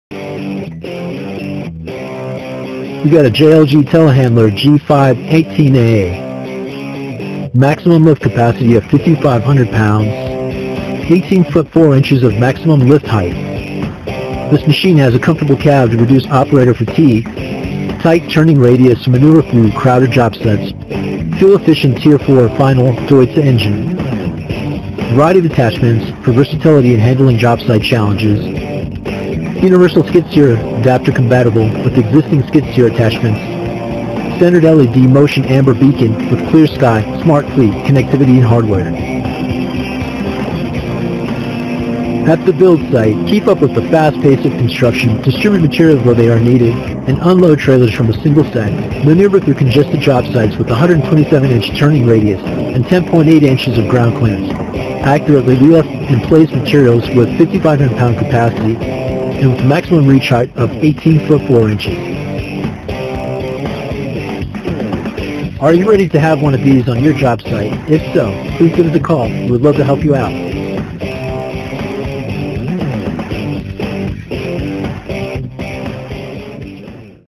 2024 JLG G5-18A For Sale
New 2024 JLG G5-18A Telehandlers (Compact)

5,500-lb Capacity • 18-ft 4-in Lift Height

Standard Features:
• Deutz TD 2.9 L4 74 hp Diesel Engine
• Hydrostatic Transmission
• Single Joystick Proportional Controls
• 4-Wheel Drive
• Three Steering Modes
• Wet Disc Service Brakes
• Adjustable Suspension Seat
• Certified ROPS/FOPS Structure
• Backup Alarm
• Glow Plugs
• Variable Speed Fan
• Auxiliary Hydraulics
• Open Cab
• Telematics-Ready Harness
• Boom-Mounted Lifting Lug

Priced In Options:
• Open Cab
• Work Light Package (Cab—Two Front, Two Rear; Two Boom, Rotating Beacon)
• Universal Skid Steer Coupler (Mechanical)
• Standard Carriage, 50 inch
• Pallet Forks (2) 2 x 4 x 48 inch
• 12 x 16.5 Foam Filled Tires